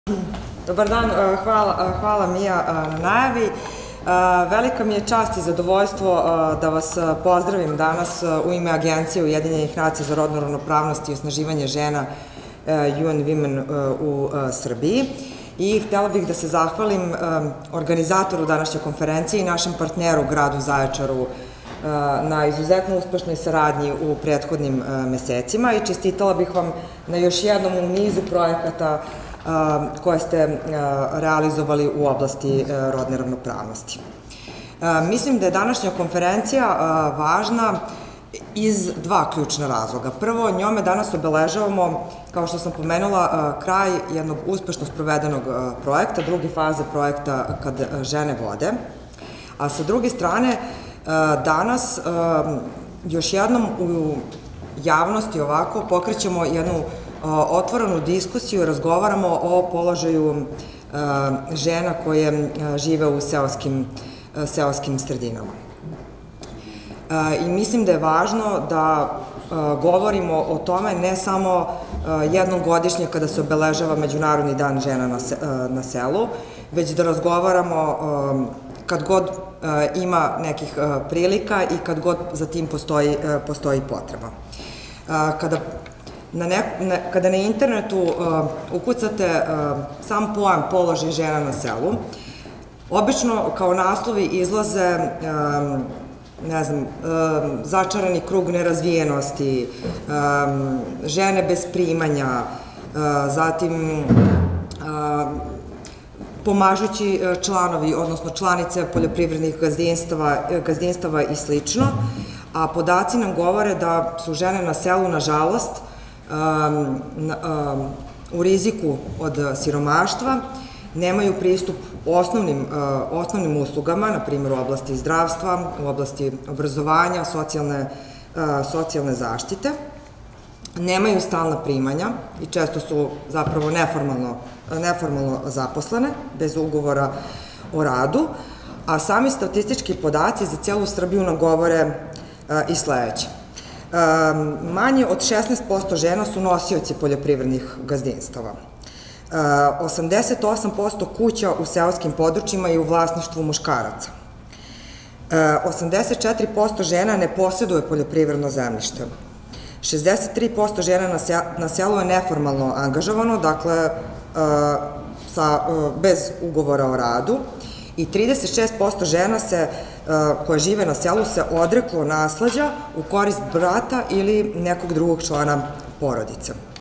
Jovana Ilic Agencija Ujedinjenih Nacija za rodnu ravnopravnost o projektu „Kad žene vode – Faza II
Povodom realizacije projekta „Kad žene vode – Faza II” u velikoj sali Gradske uprave u Zaječaru uručeni su ugovori.

Žene sa teritorije grada Zaječara dobile su opremu i materijal, kako bi unapredile i razvile seoski turizam i kako bi na taj način podigle nivo ugostiteljskih usluga u seoskim turističkim domaćinstvima, poljoprivrednim gazdinstvima ili u domaćoj radinosti.